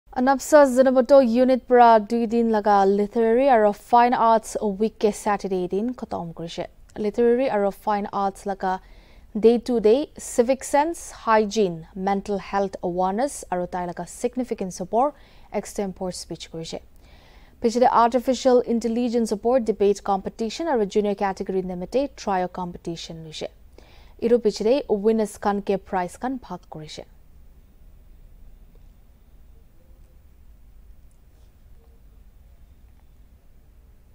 LITERARY & FINE ARTS WEEK OF ANPSA ZUNHEBOTO UNIT CONCLUDES
Literary & Fine Arts week of ANPSA Zunheboto unit concluded on August 5. Day 2 of the event started with the extempore speech on the topics such as civic sense, Hygiene, mental health awareness and its significance which was followed by debate competition on the topic AI (Artificial intelligence) and trio competition for junior category .
Followed by the price distribution:-
Debate: 1st Cornerstone hr. Sec school, 2nd: Olympic hr. Sec school, 3rd: Immanuel hr. Sec school and Cornerstone hr, sec school.
Extempore: 1st: Step by step school, 2nd: New Era school, 3rd: Olympic hr sec school.
Painting (senior) 1st: Bloomfield, 2nd: Cornerstone, 3rd: Sun beam school.
Painting (junior) 1st: Olympic,  2nd: Woodland, 3rd: Bethany school.
Painting (Elementary) 1st: Little spring, 2nd: Woodland, 3rd: New Era school.
Spelling Bee 1st: Cornerstone, 2nd: Olympic, 3rd: Woodland school.
Quartet 1st: Immanuel, 2nd: Bethany, 3rd: Olympic
Trio 1st: Immanuel, 2nd: Kids world Montessori, 3rd: Cornerstone.